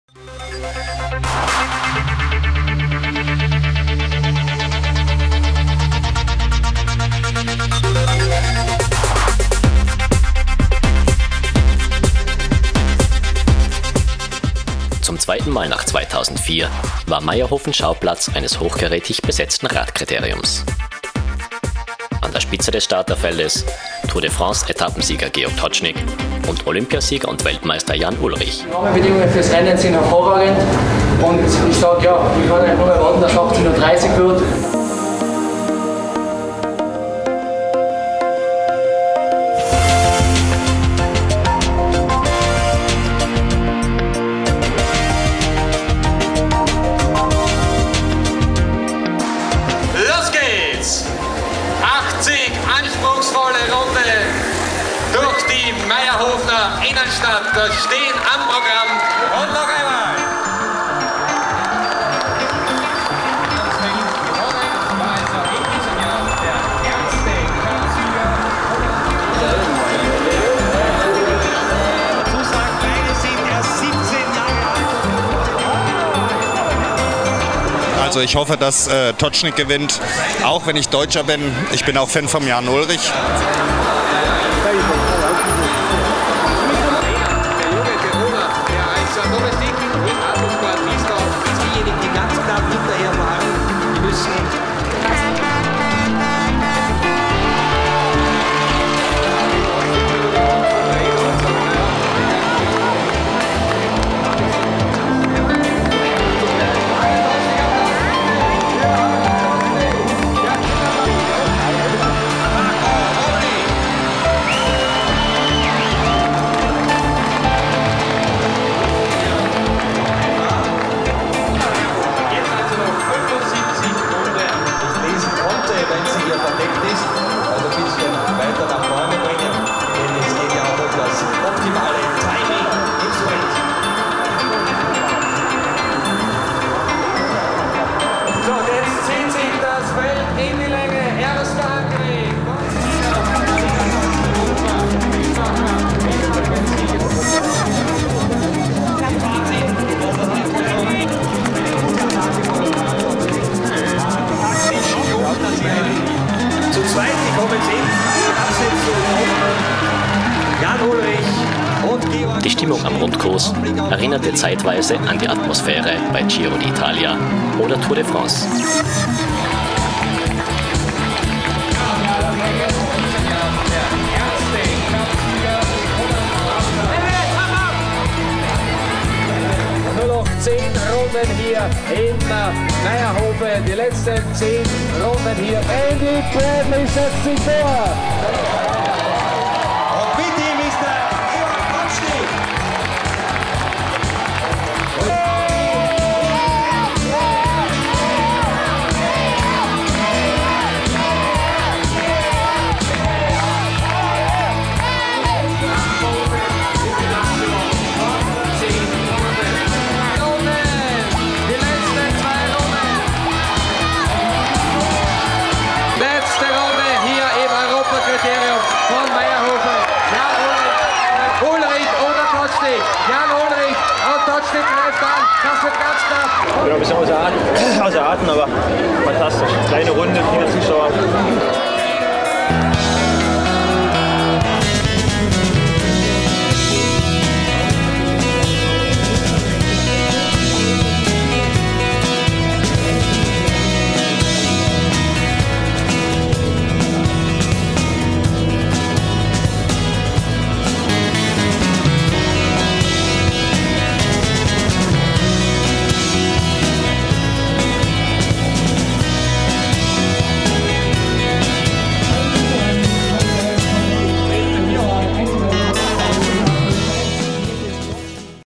Radkriterium Mayrhofen
Das Radkriterium in Mayrhofen steht für tolle Stimmung, viele Leute und unglaubliche sportliche Leistungen.